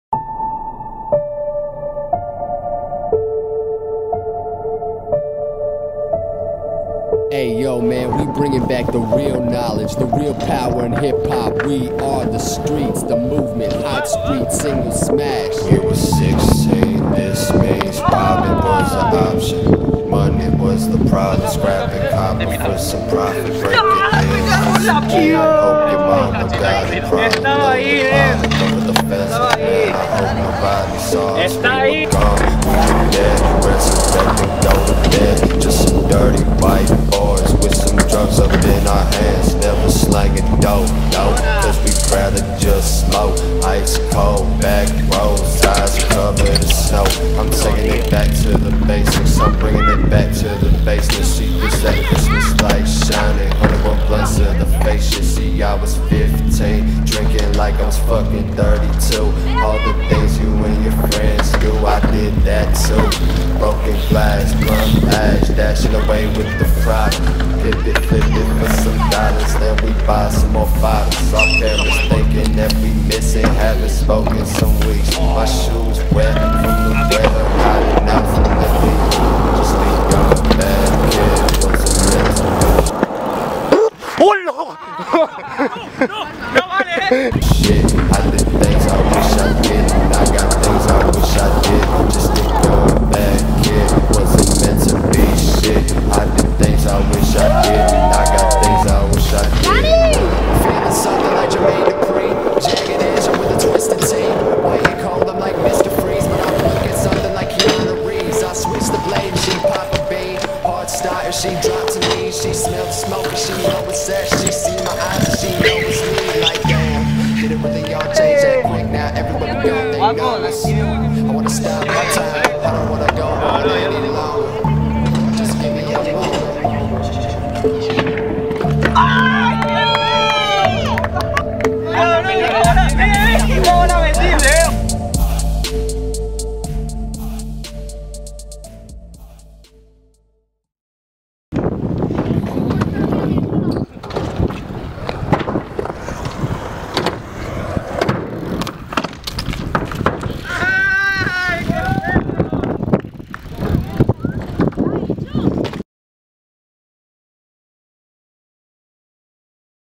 GBC Afternoons #2
Shout outs to every homie.
Video recorded in two hours.
Riders (in order): Bird H. Rider, Aion Gil, Abraham Castellano, Aleix Serra